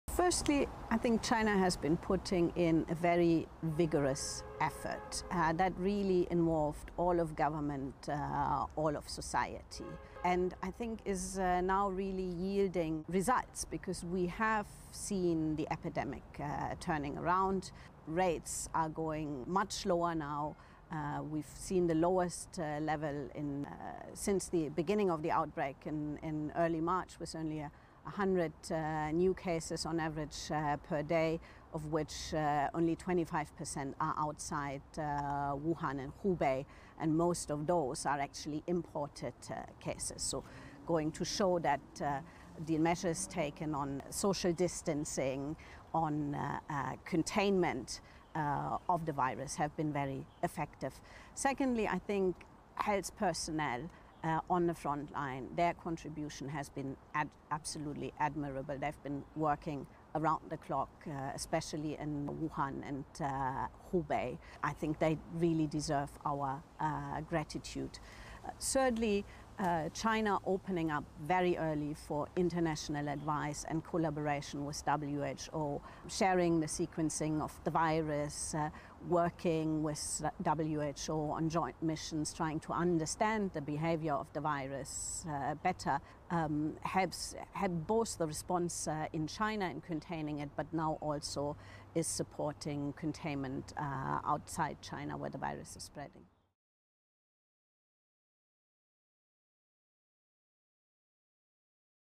UNDP representative praises measures taken in China fighting COVID-19
Beate Trankmann, the United Nations Development Programme (UNDP) resident representative in China, has praised measures taken in China to fight COVID-19. She said that previous efforts are now yielding results, as the epidemic has turned around in China with a much lower infection rate.